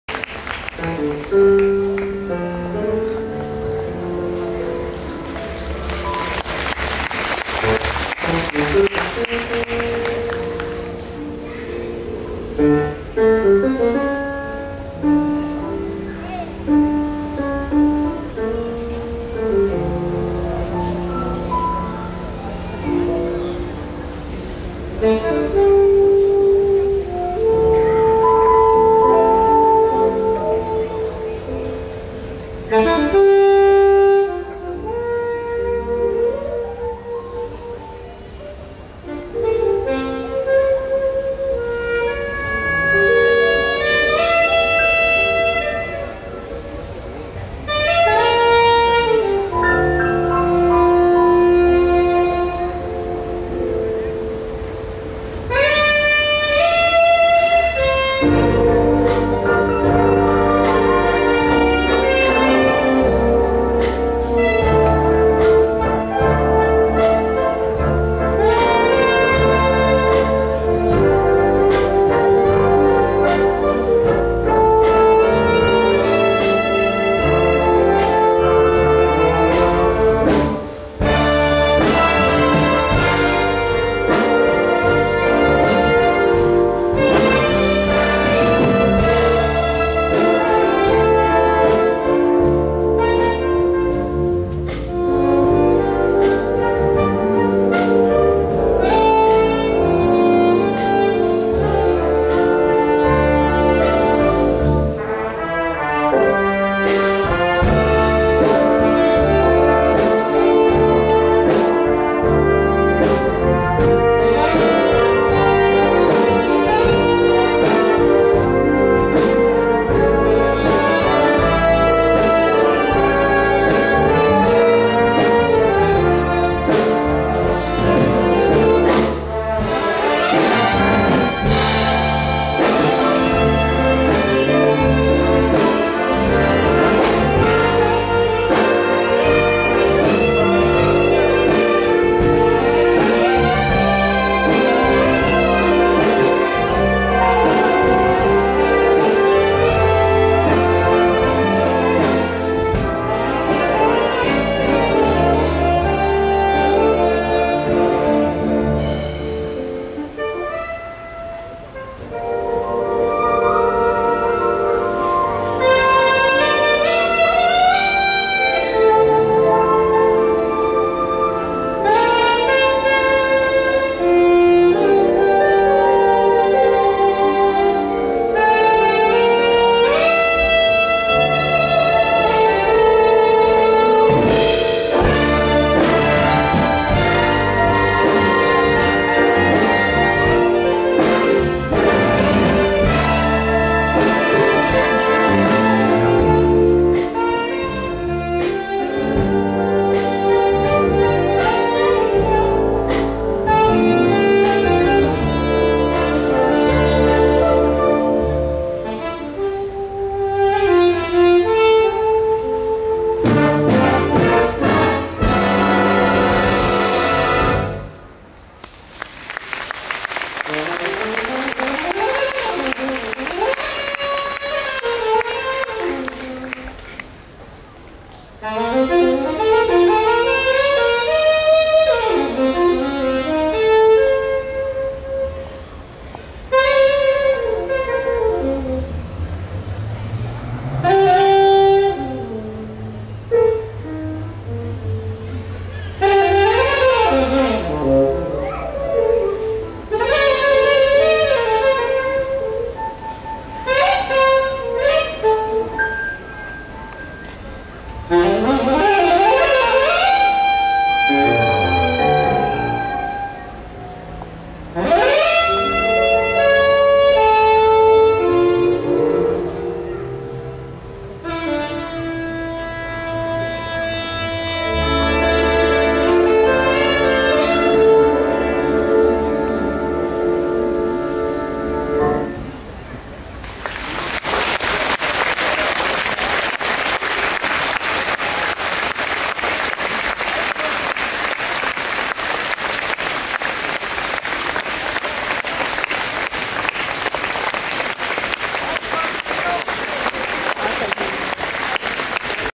Rīgas svētki 2014 Koncerts 15.08. Part.2
Vērmanes dārza estrādē Rīgas svētku ieskaņas koncertā „Rīgas vitrāžas” Profesionālais pūtēju orķestris „Rīga” saspēlēsies ar maestro Raimondu Paulu